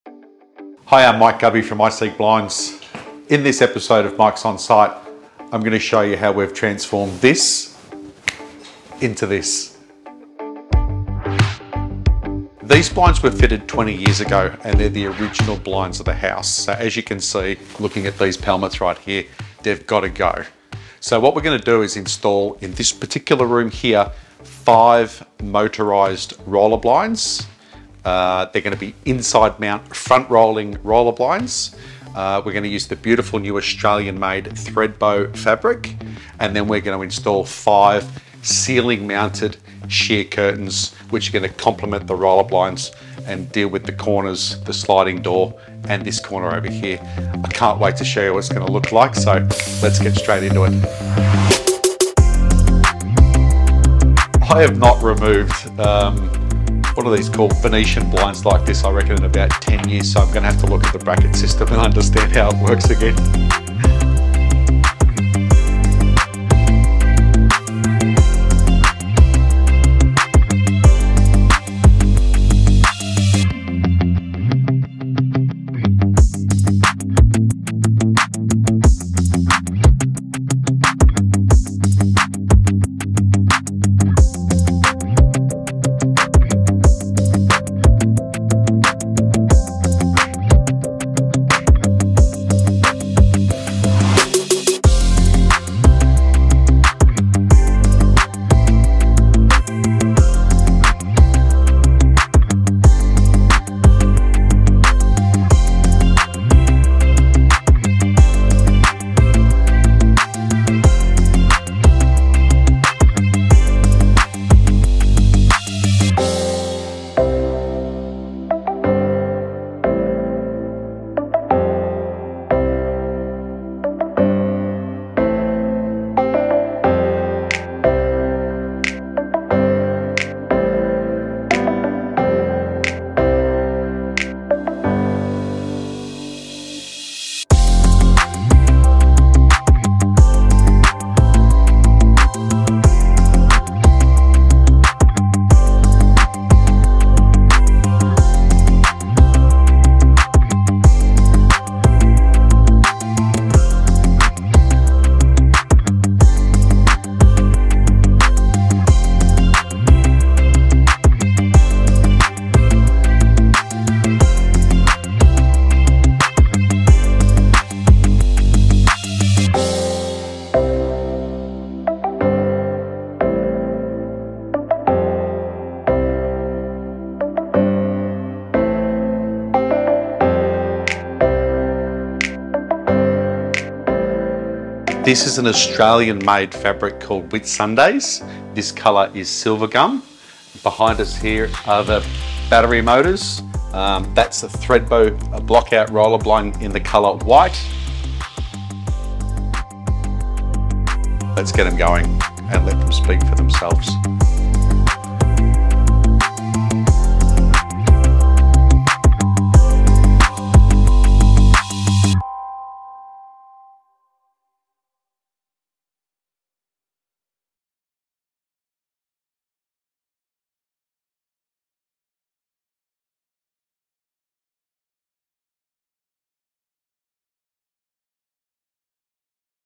Mike's Onsite - John and Liza's Home Transformation
In this episode of Mike's Onsite, Mike transforms John and Liza's home with new, motorised roller blinds and beautiful sheer curtains. Using Australian made fabrics, Mike adds a new, fresh look to the inside. Check this out to see how your own home can be transformed in a single afternoon.

To find out more about iSeekBlinds and our products, please visit us